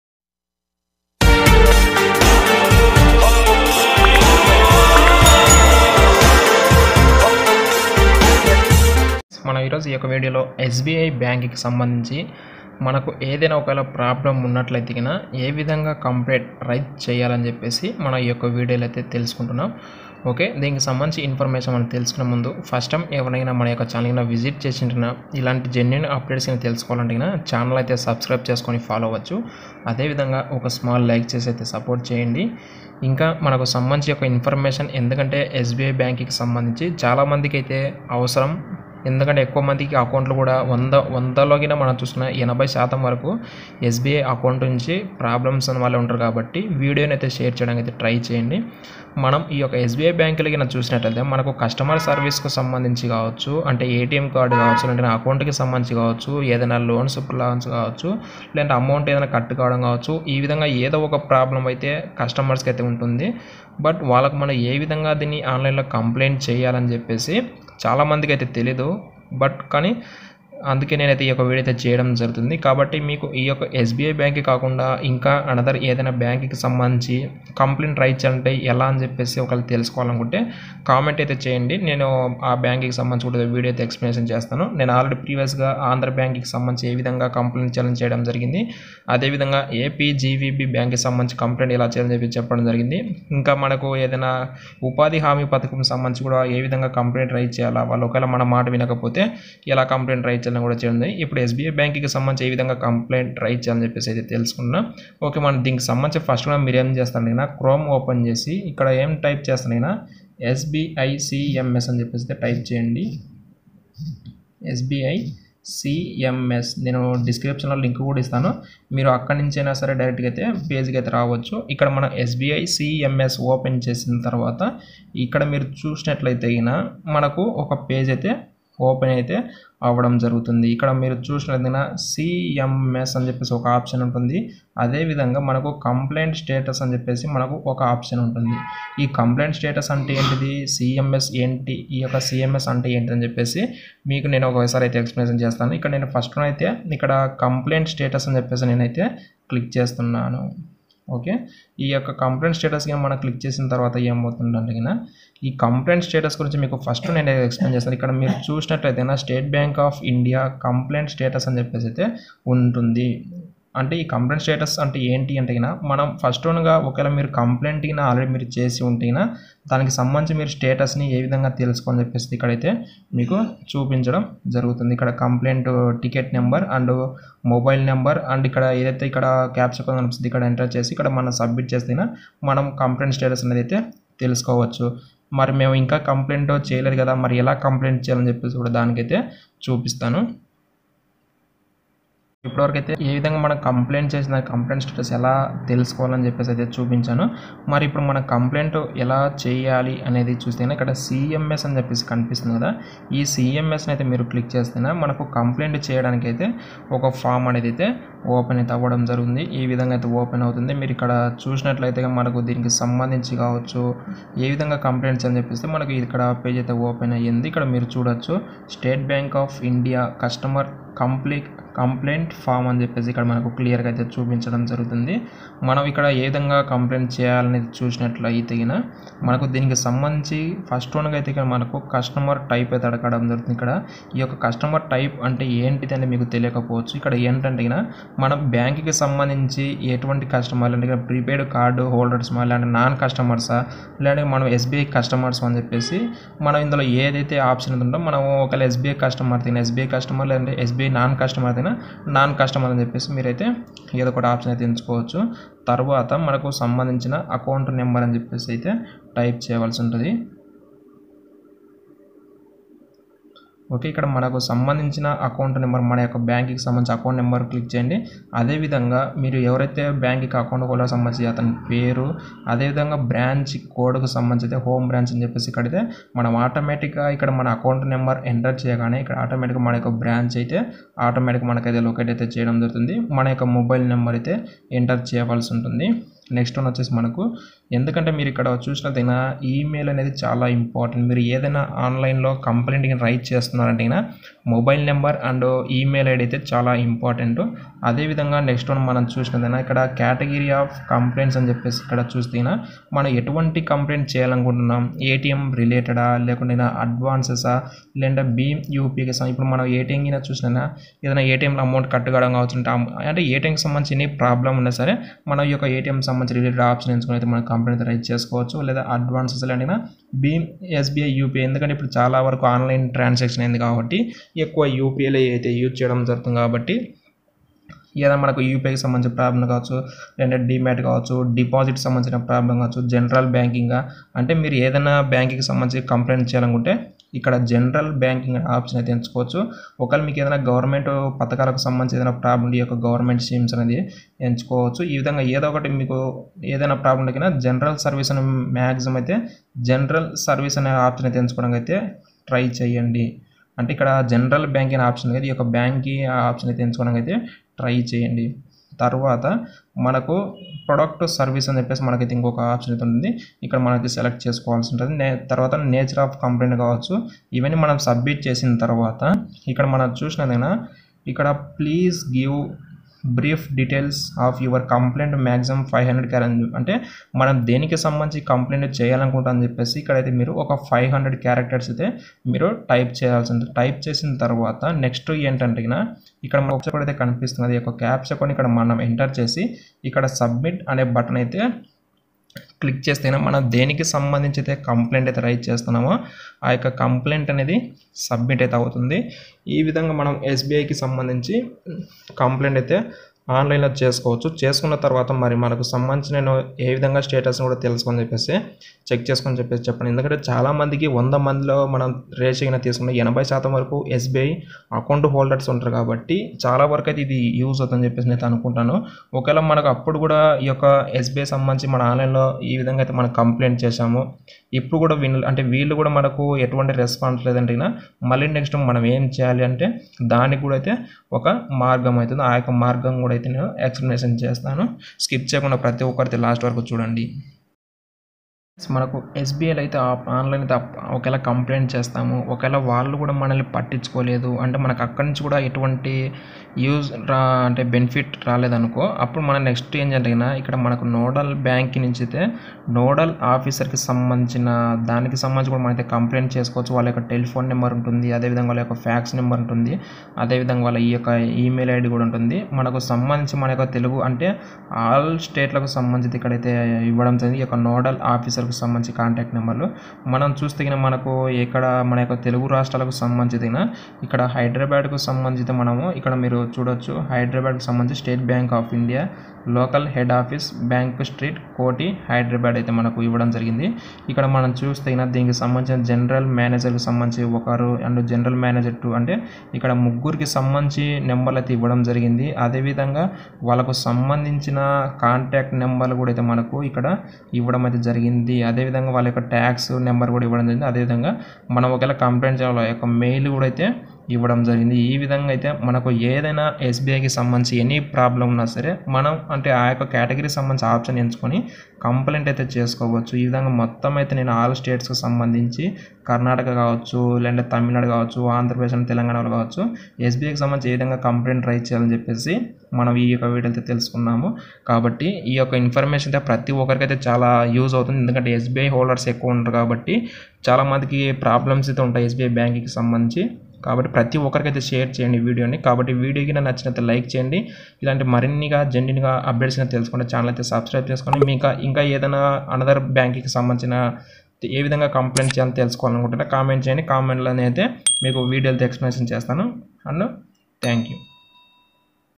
How to Raise complaint online in SBI Bank telugu.
How to raise complint online in SBI Bank telugu,how to register online complaint in SBI Bank telugu.